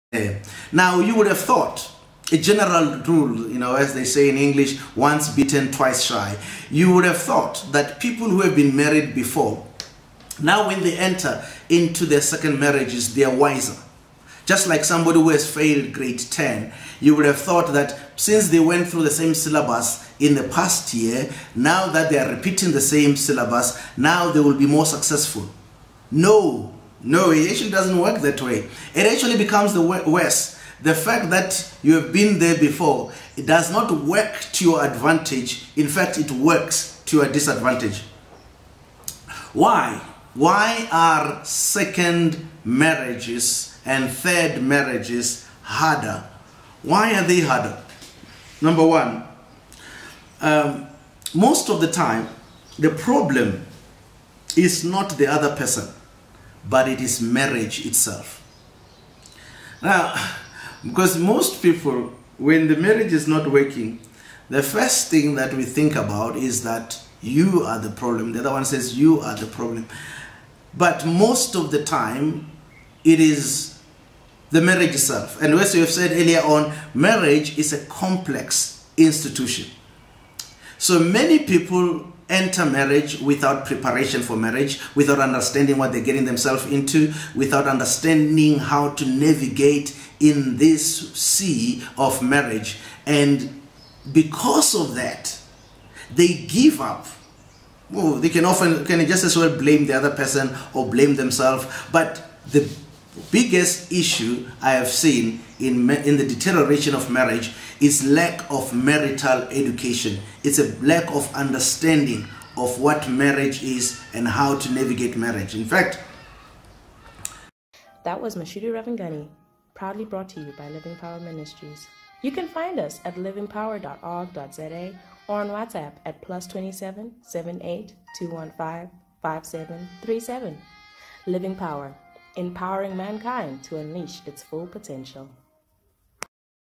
The biggest challenge in marriage is "marriage" itself by Mashudu Ravhengani
This a clip from a presentation entitled: Why 2nd and 3rd marriages are harder?